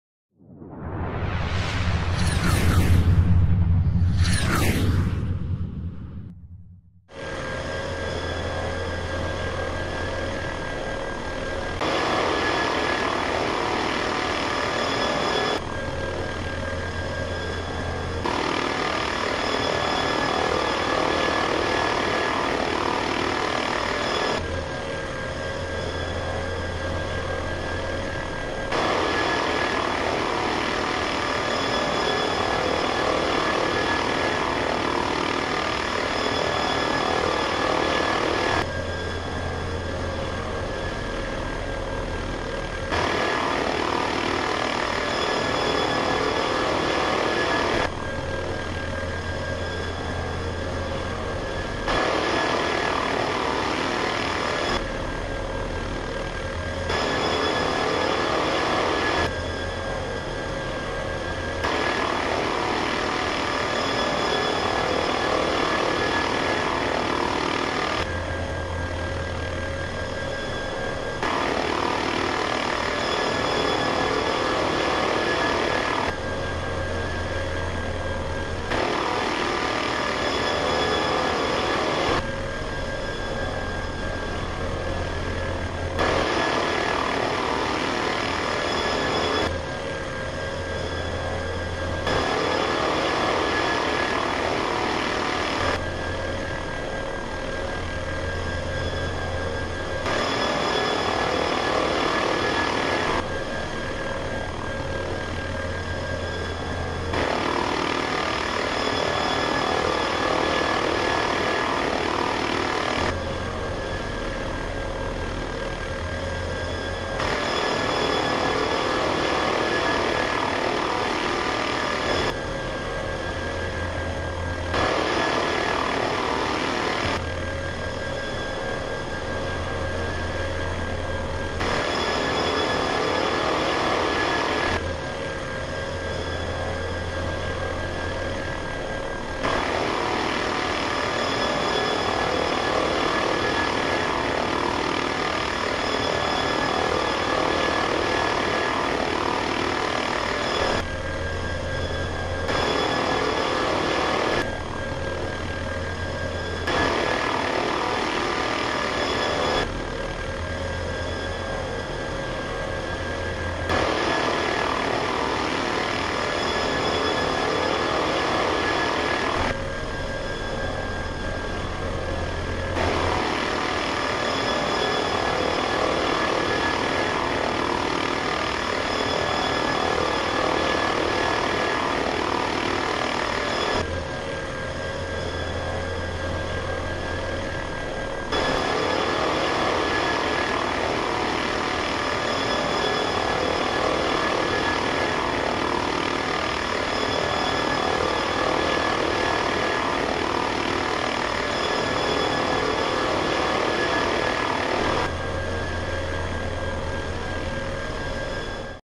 EC145 HS74 Circuit3
Scène d'entrainement pour Hélicoptère : HS74 version 2016.
Circuit des portes numéro 3.